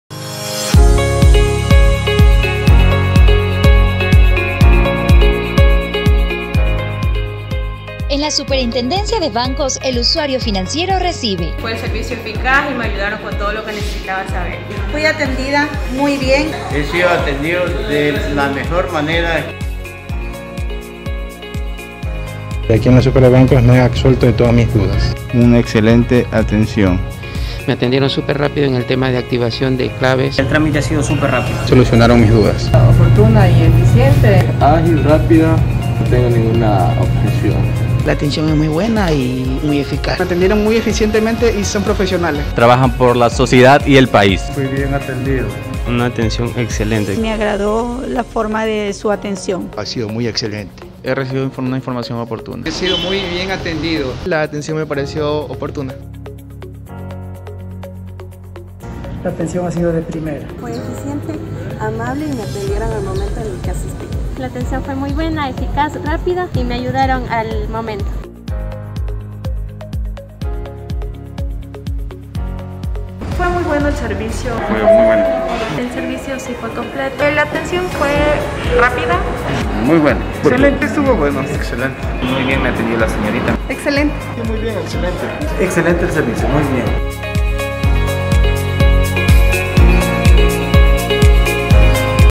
Usuarios atendidos nacional
Opinión de usuarios sobre la atención recibida en la Superintendencia de Bancos a nivel nacional.